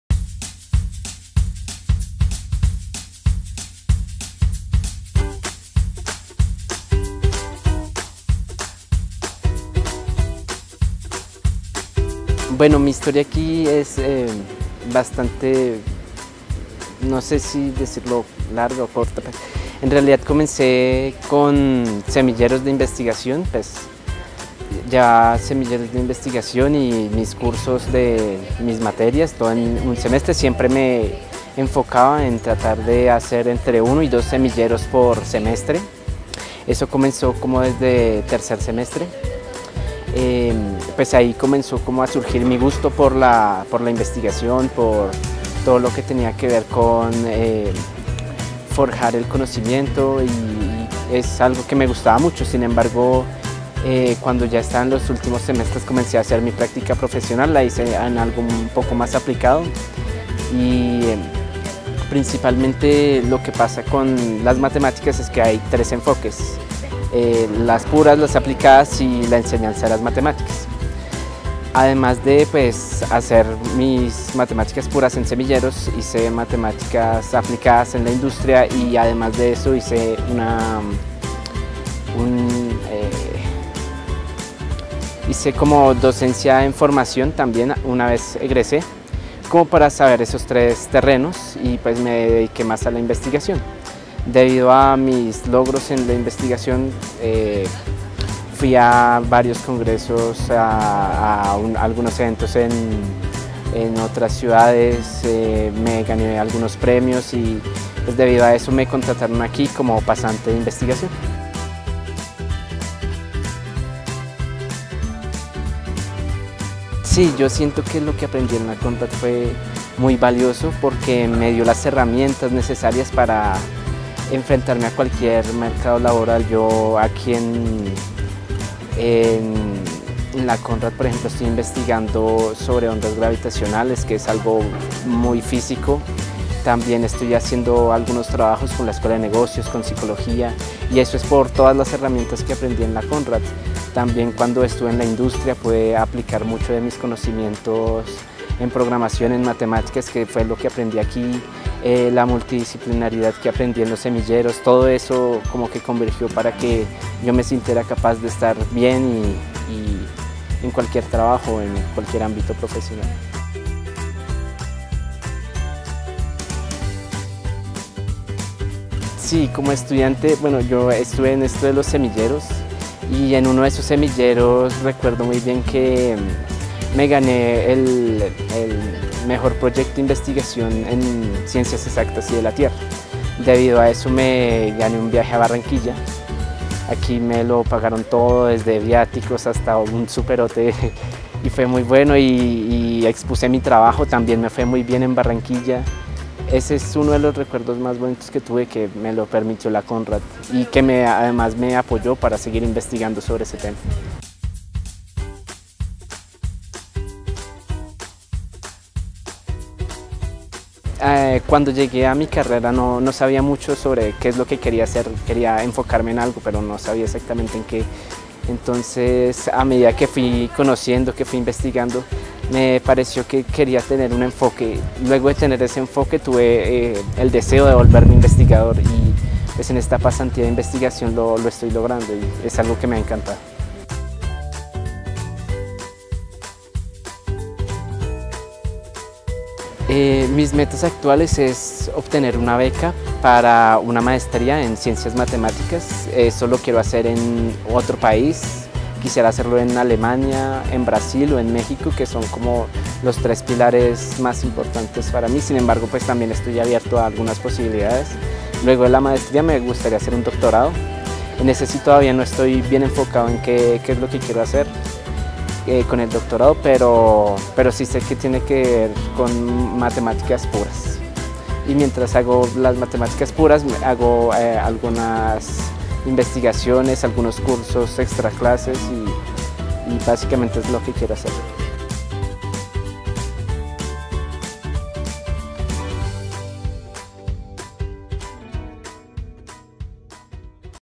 Entrevista egresado del Programa de Matemáticas
Anderson Quintero recuerda cómo fue su paso por la Konrad y cómo impactó su vida haber cursado la carrera de Matemáticas.